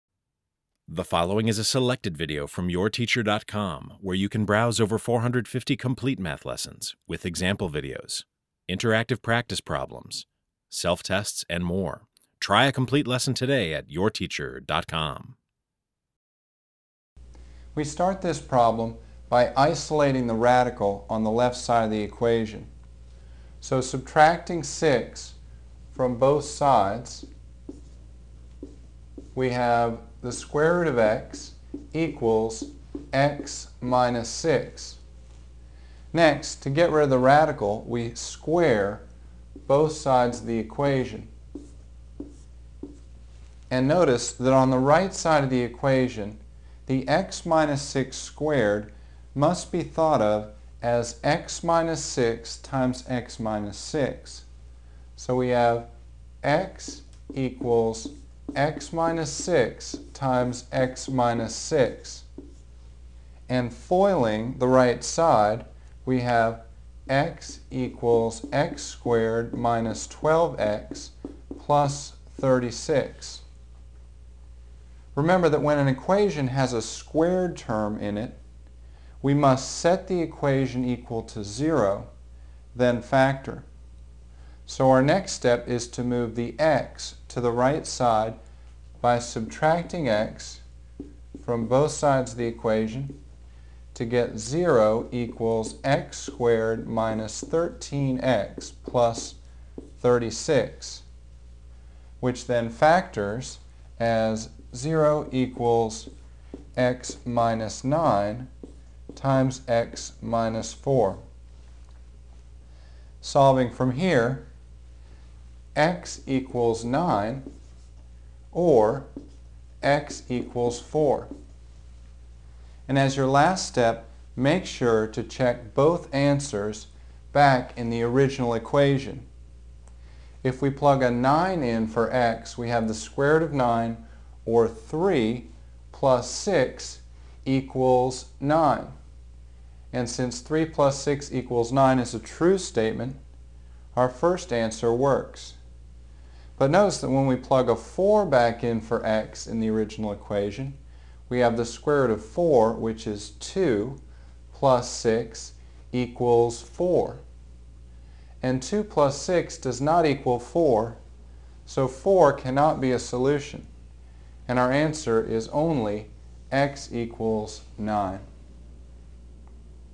Solving Radical Equations - MathHelp.com - Algebra Help
For a complete lesson on solving radical equations, go - 1000+ online math lessons featuring a personal math teacher inside every lesson! In this lesson, students learn to solve square root equations by first isolating the square root, then squaring both sides of the equation. Students also learn to solve cube root equations by first isolating the cube root, then cubing both sides of the equation. Make sure to check the solution in the original equation. If it does not check, then the answer is the null set.